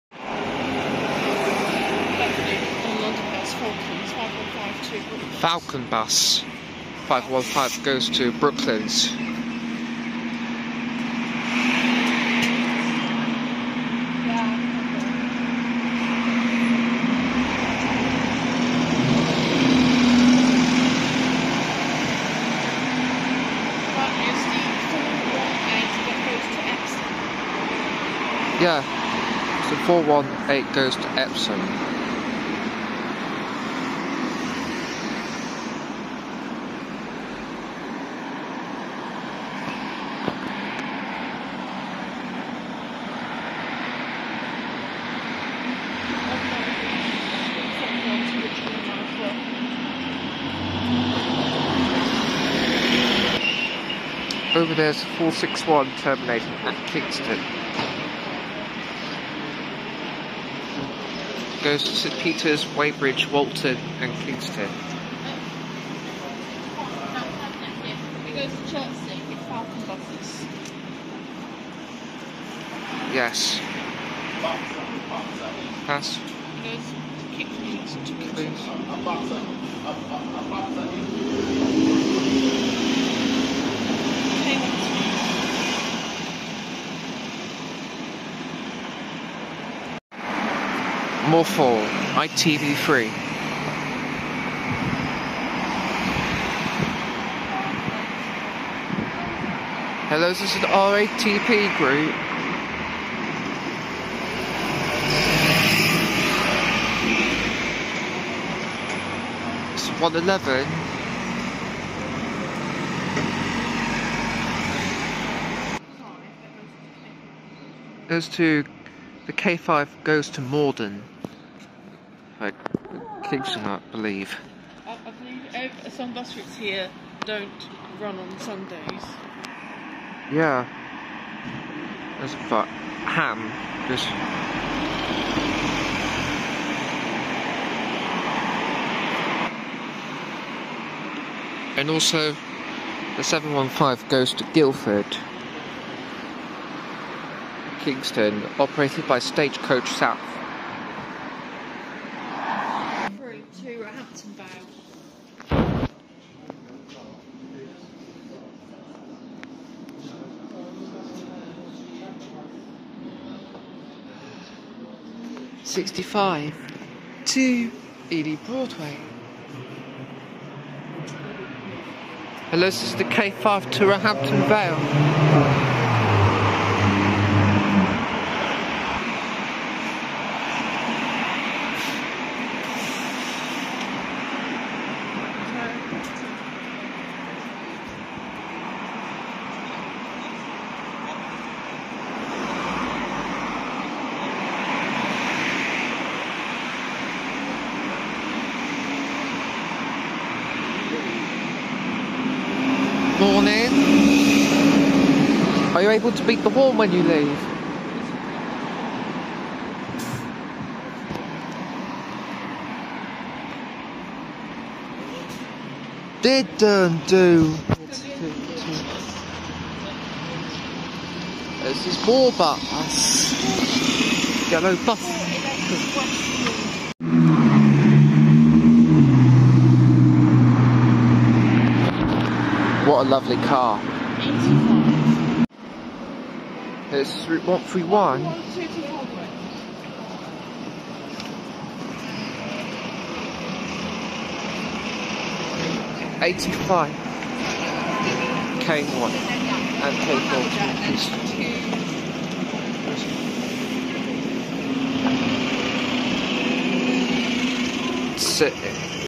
Buses & Some Vehicles In The Kingston Area Ft. Emiliano (@miahaskurti4539) (11/02/2023)
This Video Shows A Variety Of Buses And Some Vehicles In Kingston's Cromwell Road Bus Station - Mostly Operated By TFL But Some Are Not Operated By TFL.